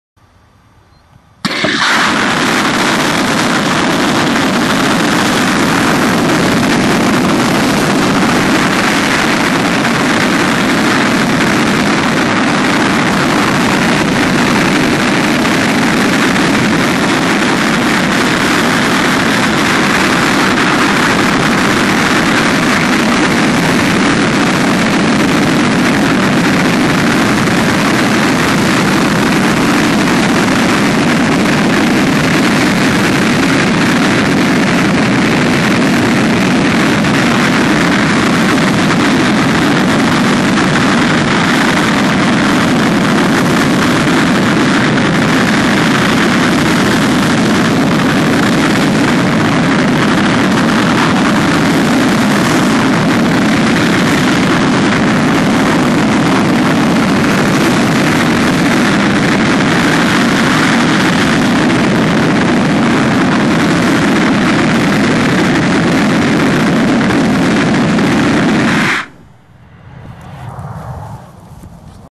Armadillo aerospace full throttle
Full throttle test of armadillo aerospace's new rocket engine